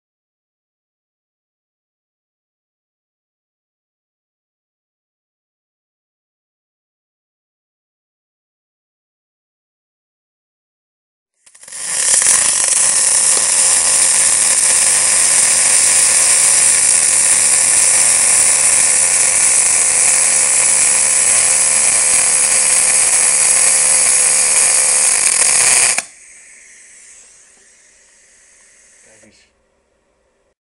MIG aluminium
Aluminium is ook te lassen met het MIG/MAG lasapparaat. Dit wordt MIG lassen genoemd omdat het beschermgas 100% Inert gas is (in dit geval Argon). Aluminium absorbeert meer warmte dan staal of RVS, er is daardoor meer stroom vereist om Aluminium te verlassen. Aluminium lasdraad is zachter dan staal on RVS lasdraad, dit vergt een draadtransportmechanisme van hogere kwaliteit en bij voorkeur een kunststof (bv. teflon) binnengeleider in het slangenpakket . In dit filmpje wordt een buitenhoek gelast met de Cebora Tri Star 1635/M.